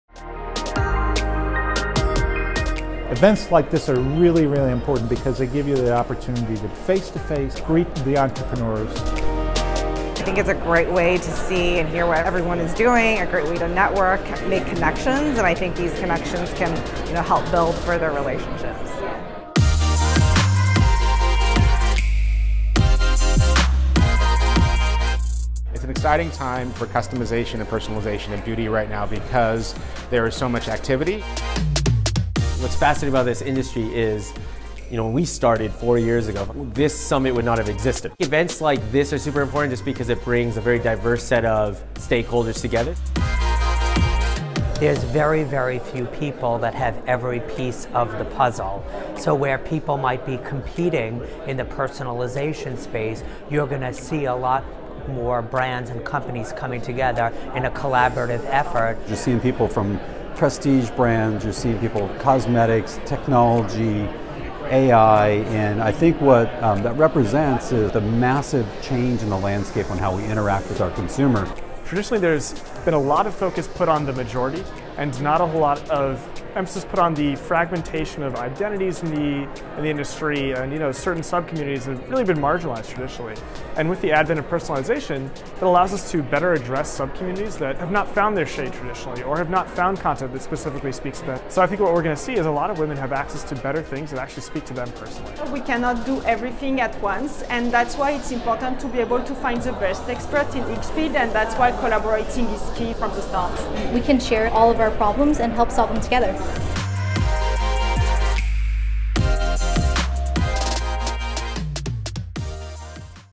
Personalized Beauty Summit USA 2019
The Personalized Beauty Summit brings together leaders from both the technology and cosmetics industry, to showcase cutting edge research and new technological advances whilst exploring key trends and consumer priorities.